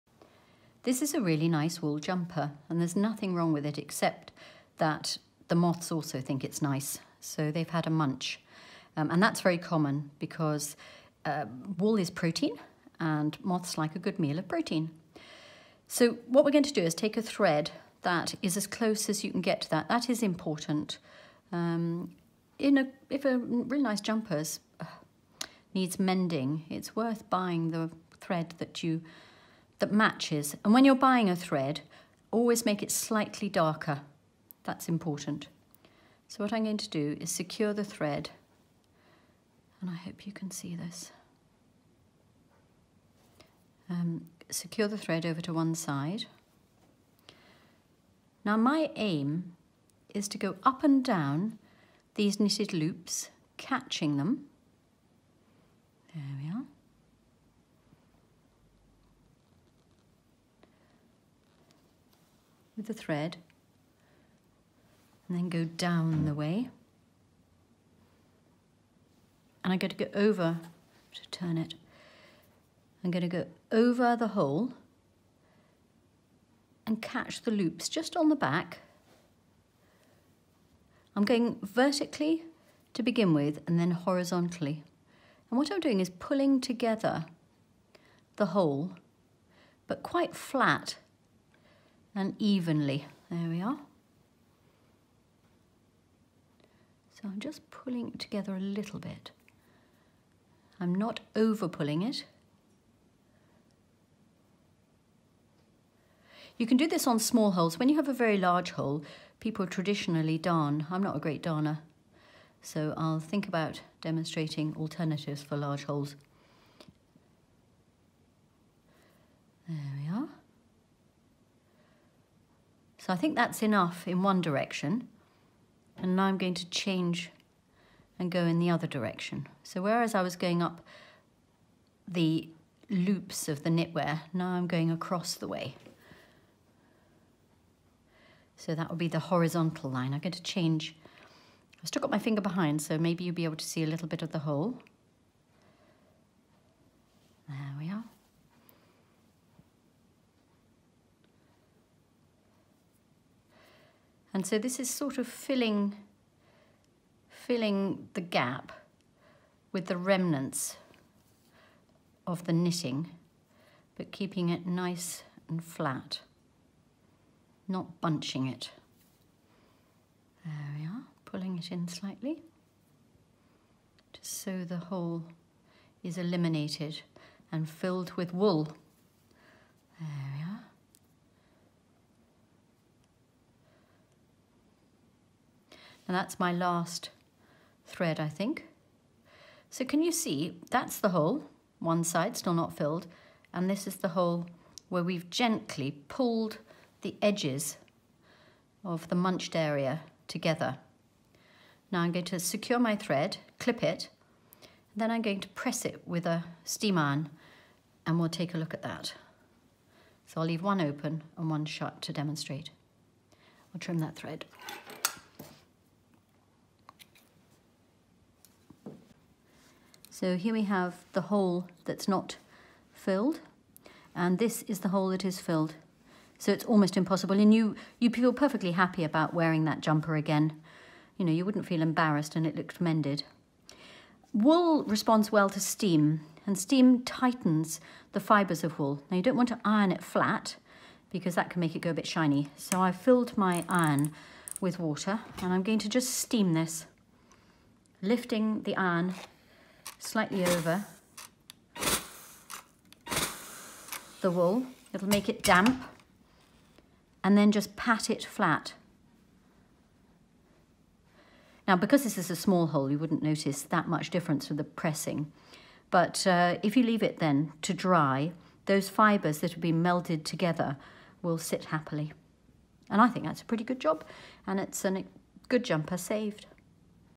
How to mend small holes in jumpers
Often caused by moths or a pulled thread, most holes can be closed and almost invisible - just don't put it off as the larger a hole gets, the harder it is to repair! This video talks through how to mend moth holes in jumpers, with some useful tips on thread colouring and all-important technique.

We also have a video on repairing underarm holes which can be trickier - watch it